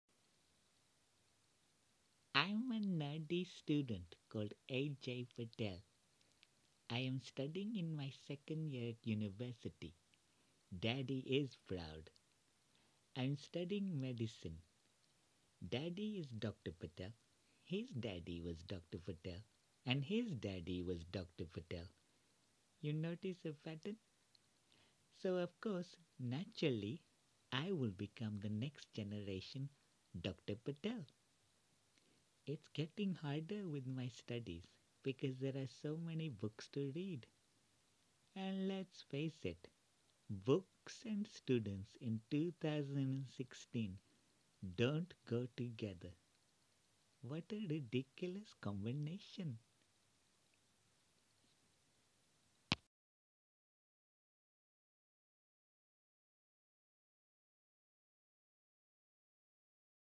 Comedy Podcast - Nerdy Student by Rahim Galia
Intro:

Awkward nerdy medical Student who is following in his father's footsteps by becoming the next Doctor Patel.

Voiced & Written by Rahim Galia
Illustration by Alex Waeland